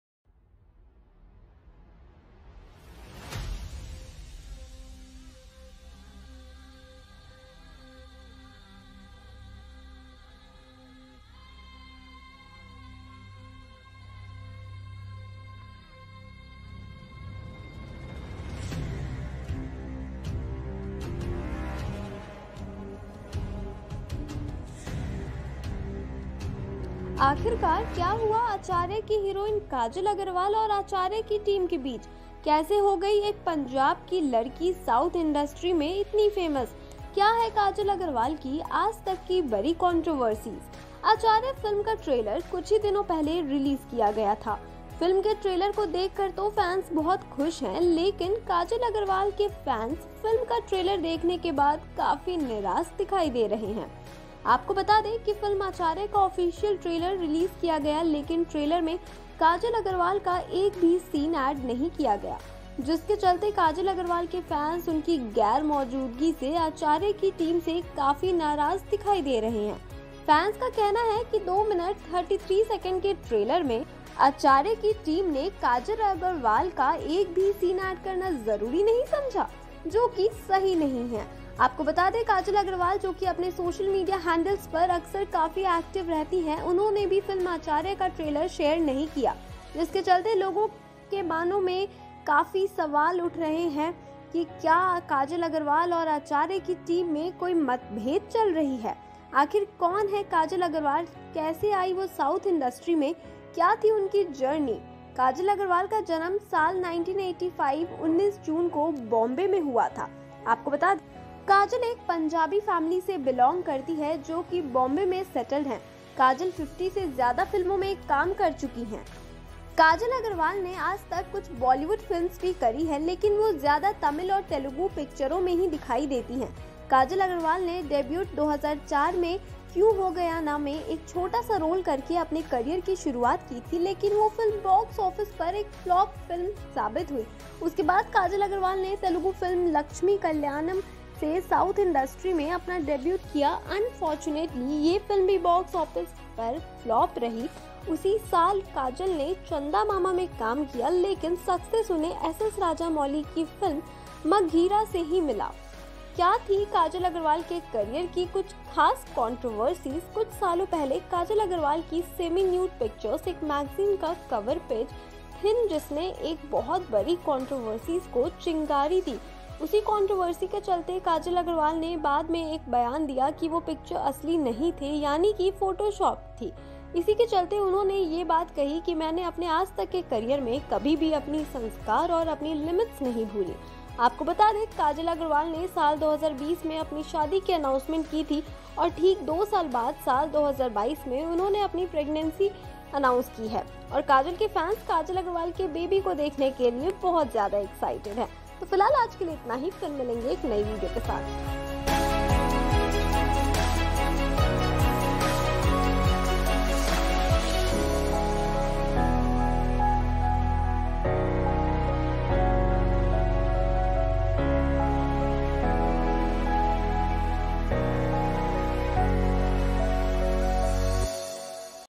"AcharyaTrailer" Official |51 Interesting facts |SalmanKhan|Chiranjeevi |Koratala Siva|Ram Charan
Acharya is an upcoming Indian Telugu-language action drama film written and directed by Koratala Siva.

Key Questions:-
0:00 INTRO
0:27 AAKHIRKAAR KYA HUA ACHARYA KI HEROINE KAJAL AGGARWAL AUR ACHARYA KI TEAM KE BEECH?
0:32 KAISE HO GAYI EK PUNJAB KI LADKI SOUTH-INDUSTRY MEIN ITNI FAMOUS?
0:37 KYA HAIN KAJAL AGGARWAL KI AAJ TAK KI BADI CONTROVERSIES?

It is produced by Matinee Entertainment and Konidela Production Company. The film stars Chiranjeevi, Ram Charan, Pooja Hegde and Kajal Aggarwal. It features music composed by Mani Sharma with cinematography by Tirru.

The film was launched in October 2019. Principal photography began in January 2020 but the production has been delayed owing to the COVID-19 pandemic. The film is scheduled for a worldwide release on 29 April 2022

The film was launched on 8 October 2019 on the occasion of Vijayadasami tentatively titled as Chiru152. The film marks the maiden collaboration of Chiranjeevi with the director Koratala Siva. The title was accidentally revealed as Acharya by Chiranjeevi during audio launch of the film O Pitta Katha on 1 March 2020.

Mani Sharma is composing the film's music. Cinematography and editing are performed by Tirru and Naveen Nooli respectively. Suresh Selvarajan is the art director.

In January 2022, Asianet News reported that the film is based on the Naxalite movement in the Srikakulam district led by poet and cultural activist Subbarao Panigrahi.

Must watch -

Anchor= Prachi
Editor = Simran Wakalkar
Writer= Masumi

Thumbnail :-KYA KAJAL AGGARWAL NAHIN RAHI ACHARYA KA HISSA?

Uploaded By- Deepika Singhal